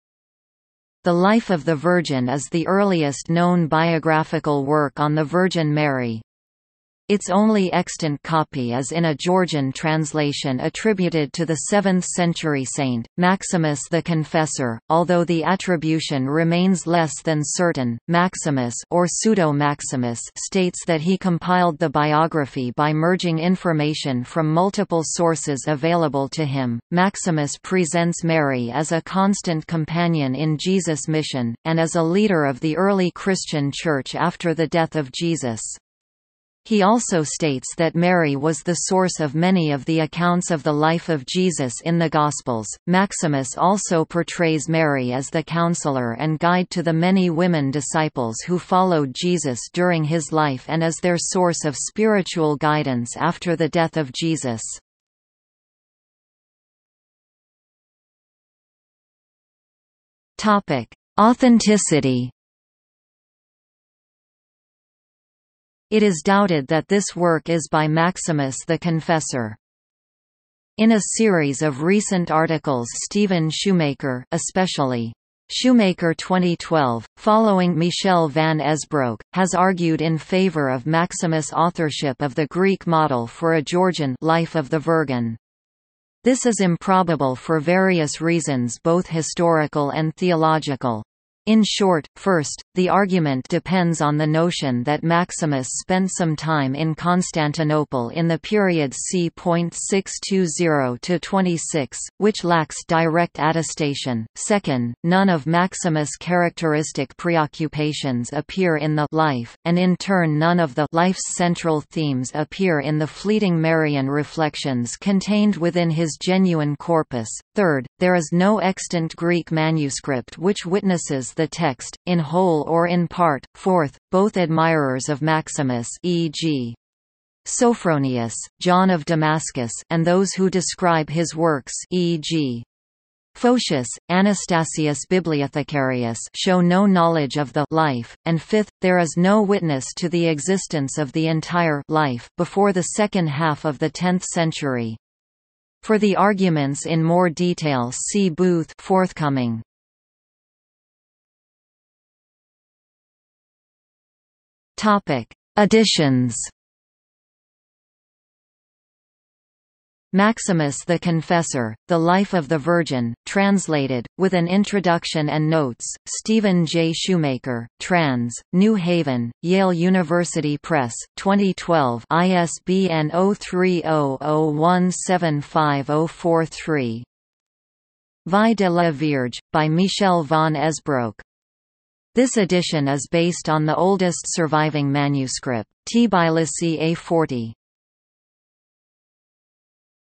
Life of the Virgin (Biography) | Wikipedia audio article
00:00:56 Authenticity
00:02:31 Editions

- Socrates

SUMMARY
The Life of the Virgin is the earliest known biographical work on the Virgin Mary. Its only extant copy is in a Georgian translation attributed to the seventh-century saint, Maximus the Confessor, although the attribution remains less than certain.Maximus (or Pseudo-Maximus) states that he compiled the biography by merging information from multiple sources available to him.Maximus presents Mary as a constant companion in Jesus' mission, and as a leader of the early Christian Church after the death of Jesus. He also states that Mary was the source of many of the accounts of the life of Jesus in the Gospels.Maximus also portrays Mary as the counselor and guide to the many women disciples who followed Jesus during his life and as their source of spiritual guidance after the death of Jesus.